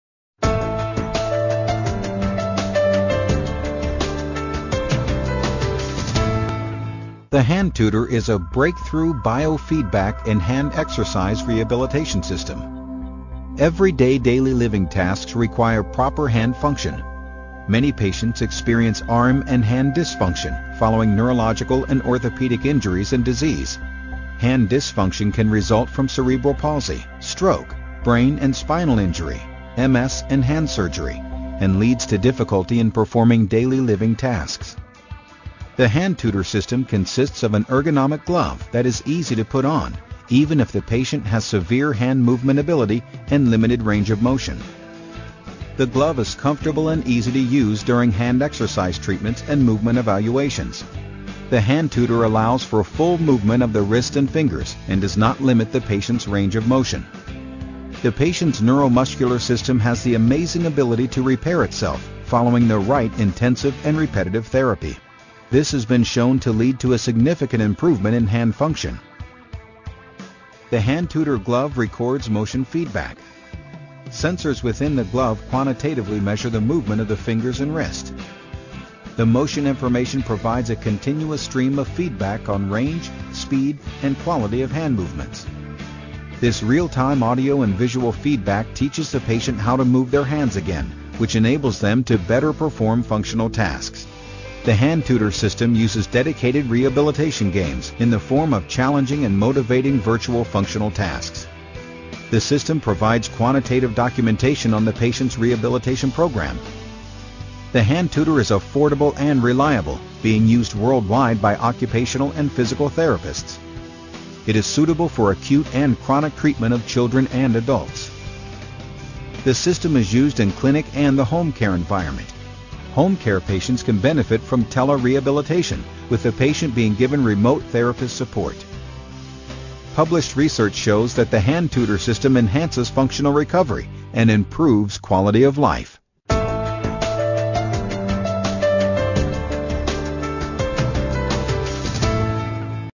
HandTutor: Αποκατάσταση λεπτής κίνησης άνω άκρων
Παρουσίαση συστήματος HandTutor για αποκατάσταση (αξιολόγηση και επανεκπαίδευση) λεπτής κίνησης άνω άκρων με διαδραστικό γάντι και ειδικό λογισμικό

Το σύστημα HandTutor είναι μέρος μίας ολοκληρωμένης σειράς διαδραστικών συστημάτων εικονικής αποκατάστασης που περιλαμβάνει επίσης το LegTutor για γόνατο και ισχίο, το ArmTutor για αγκώνα και ώμο, το 3DTutor για πληθώρα σημείων του σώματος, όπως κορμός, αυχένας, βραχίονας, καρπός, μηρός, ποδοκνημική, κ.α.

Περισσότερες πληροφορίες για Ελλάδα και Κύπρο μπορείτε να βρείτε στη σελίδα της εταιρείας μας Ostracon στη διεύθυνση ή επικοινωνώντας μαζί μας στο .